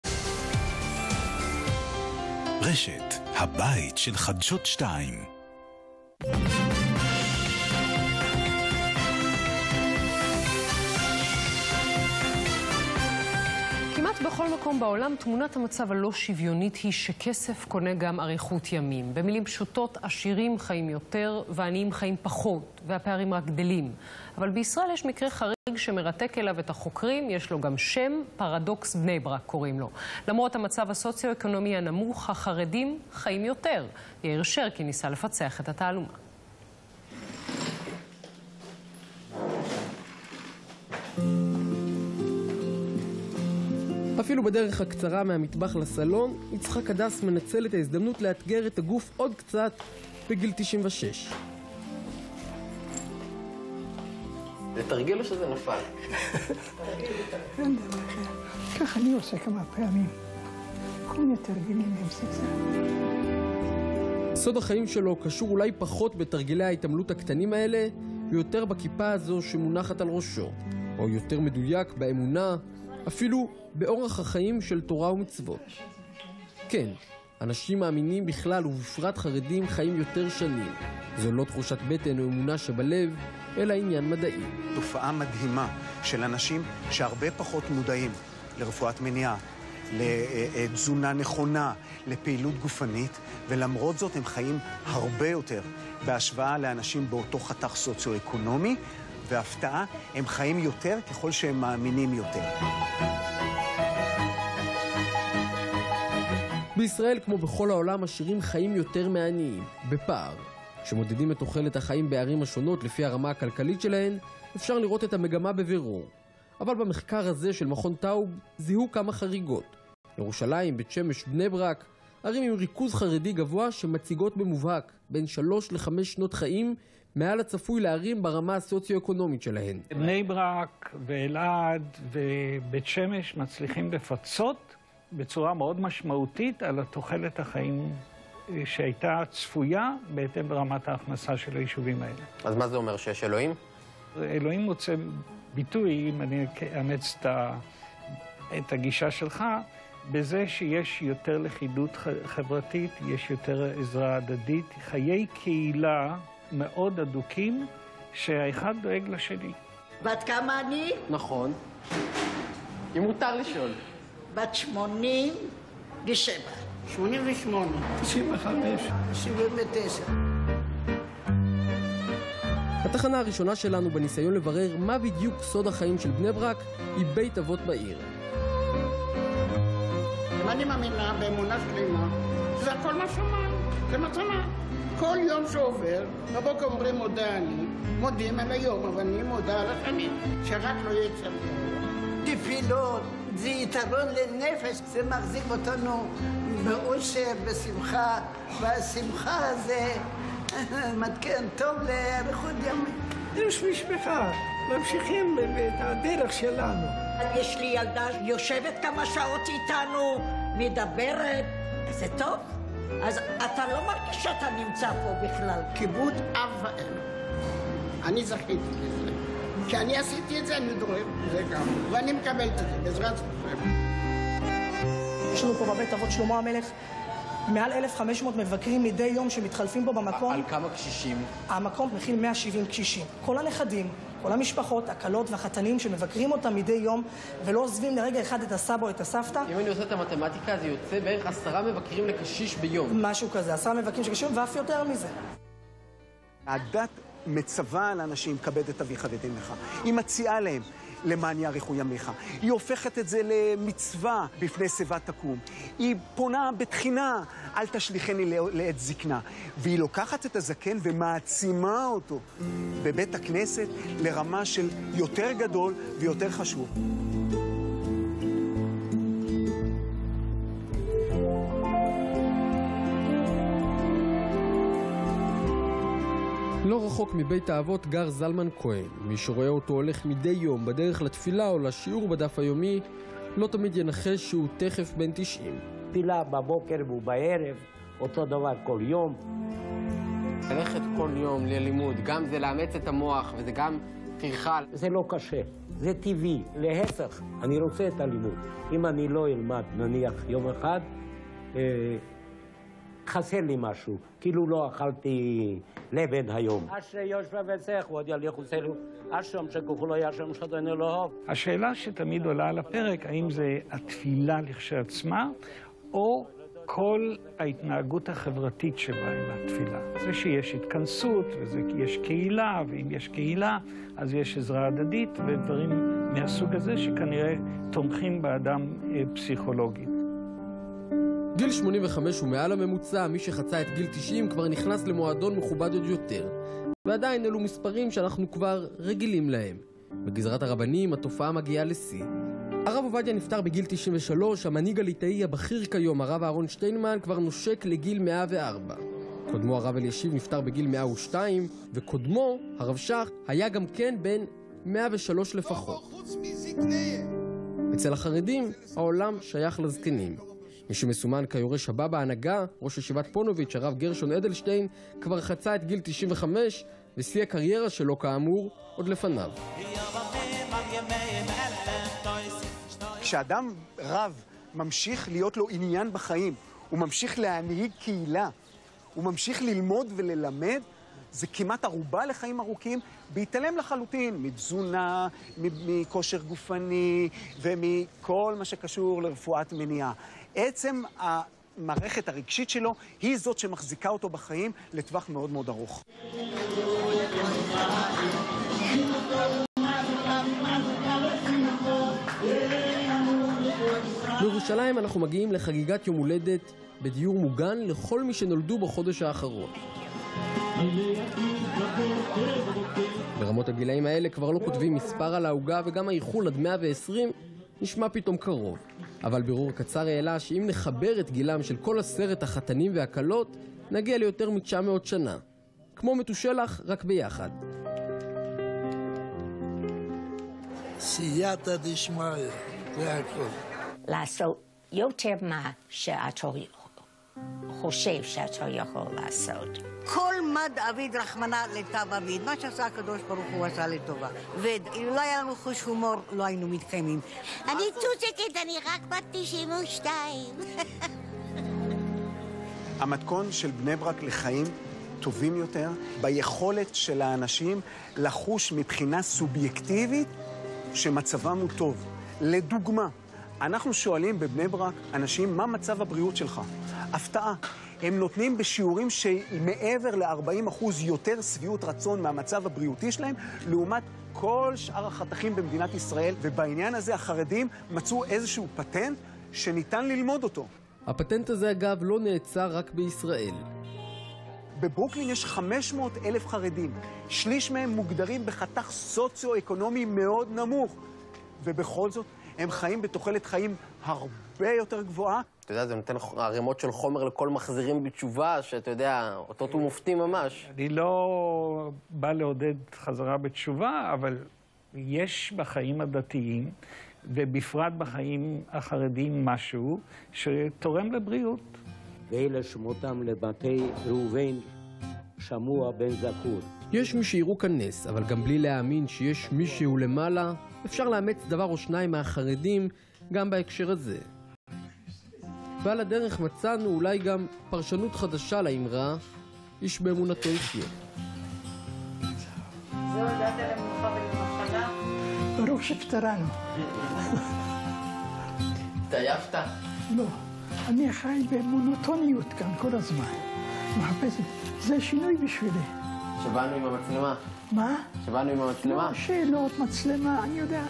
מהו סוד אריכות החיים של החרדים?
חדשות ערוץ 2 בכתבה בעקבות המחקר שמראה שתוחלת החיים בישובים חרדים גבוהה מהצפוי בהתחשב במצבם הכלכלי והחברתי. בין המרואיינים: דב צ'רניחובסקי